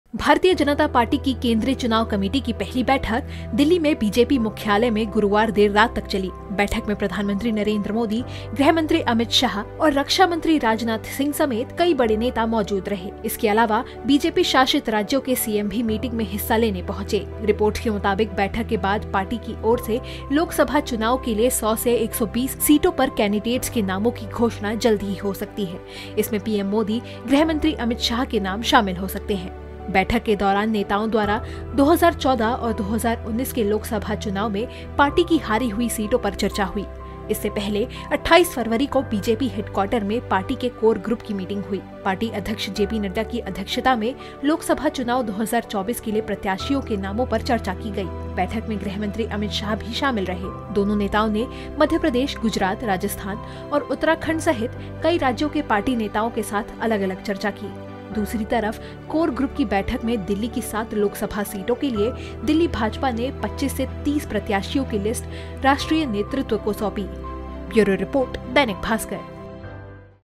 BJP Candidate List 2024: MP की 29 लोकसभा सीटों के प्रत्याशियों पर मंथन|Loksabha Election 2024
भारतीय जनता पार्टी की केंद्रीय चुनाव कमेटी (CEC) की पहली बैठक दिल्ली स्थित BJP मुख्यालय में गुरुवार देर रात तक चली। करीब 4 घंटे चली बैठक में प्रधानमंत्री नरेंद्र मोदी, गृह मंत्री अमित शाह और रक्षा मंत्री राजनाथ सिंह समेत शीर्ष नेता मौजूद रहे। इसके अलावा BJP शासित राज्यों के CM भी मीटिंग में हिस्सा लेने पहुंचे।